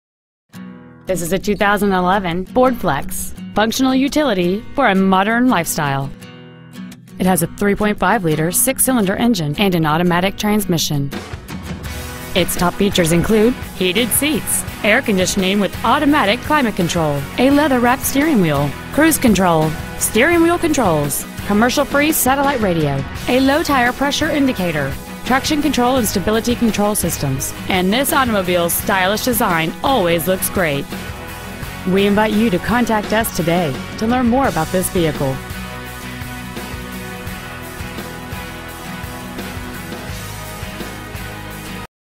Preowned 2011 Ford Flex Chattanooga TN
Year: 2011
 Make: Ford
 Model: Flex
 Engine: 3.5L V6
 Trans.: Automatic
 Exterior: Red

 Interior: Gray